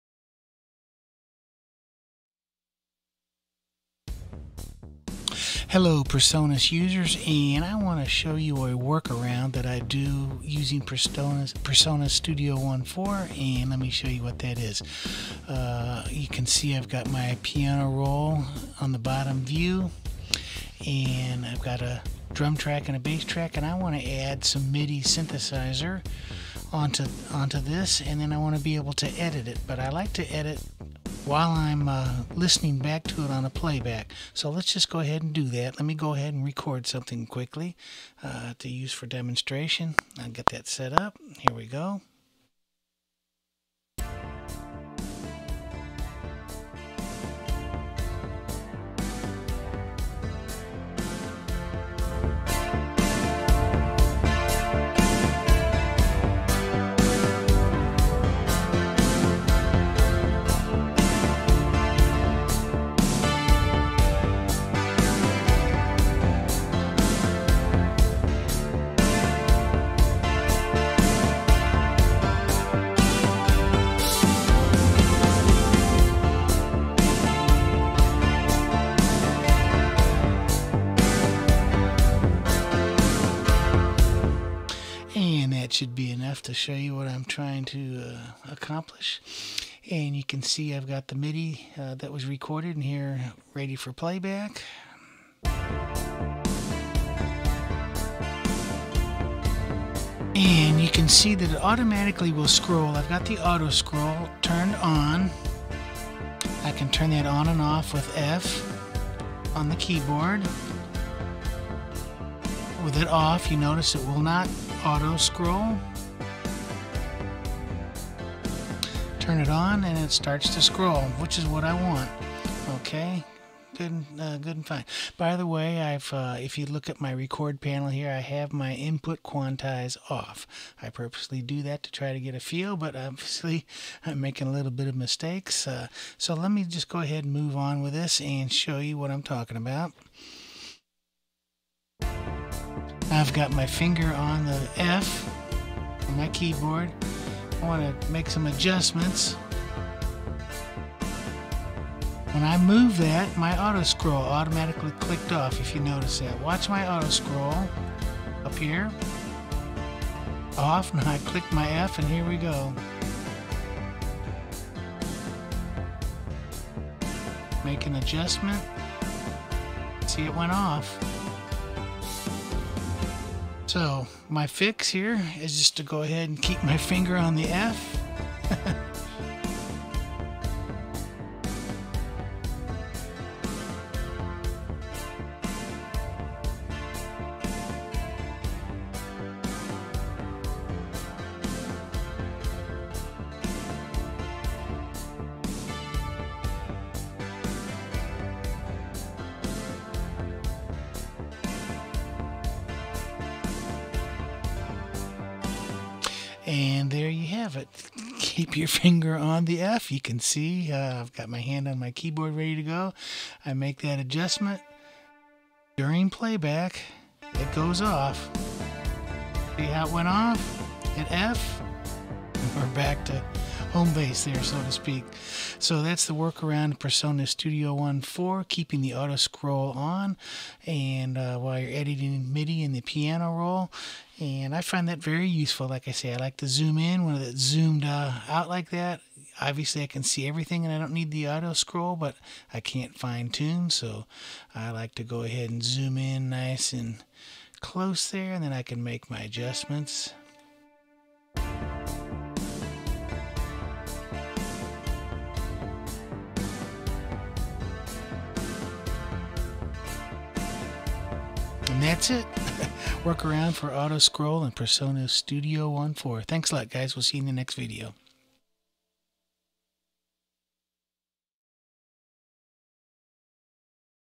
PreSonus Studio One - QUICK TIP - fast editing
How to overcome a scrolling deficiency in Studio One.  I learned this in the forums the other day (thanks Rocco!) and it helps immensely.  Edit ON THE FLY and keep the view window scrolling.
HERE'S HOW